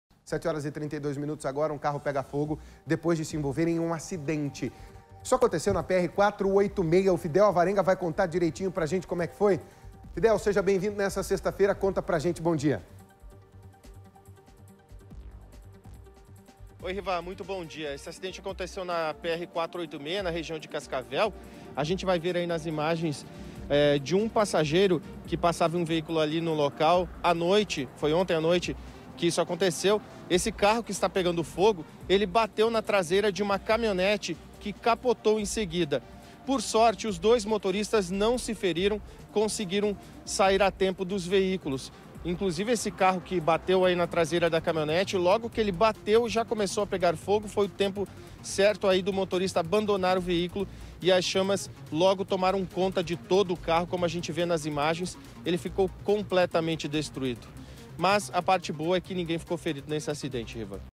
Carro pega fogo depois de bater em uma camionete na PR-486
Um carro pegou fogo depois de se envolver em um acidente. Confira todos os detalhes na reportagem!